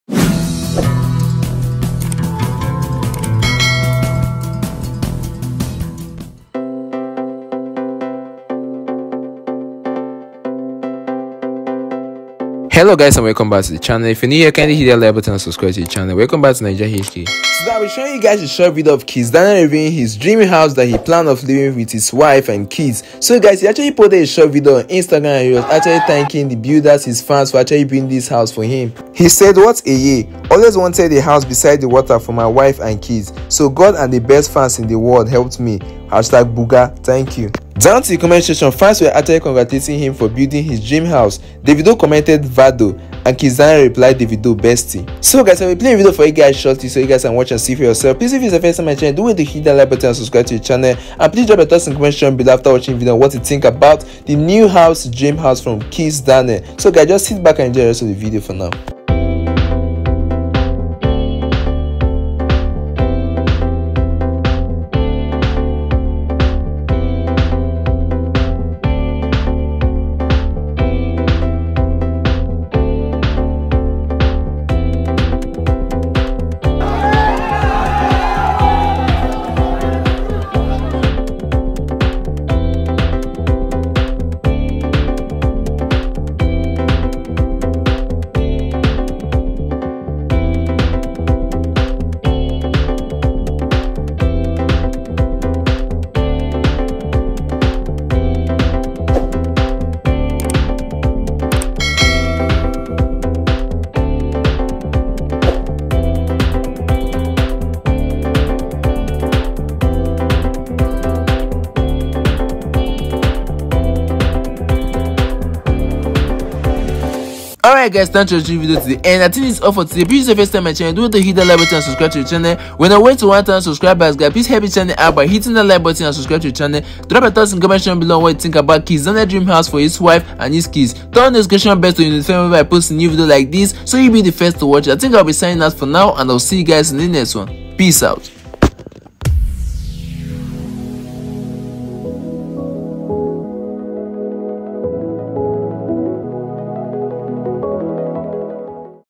Kizz Daniel Review His | Dream House Worth Over $5Million!
Kizz Daniel Review His | Dream House Worth millions of dollars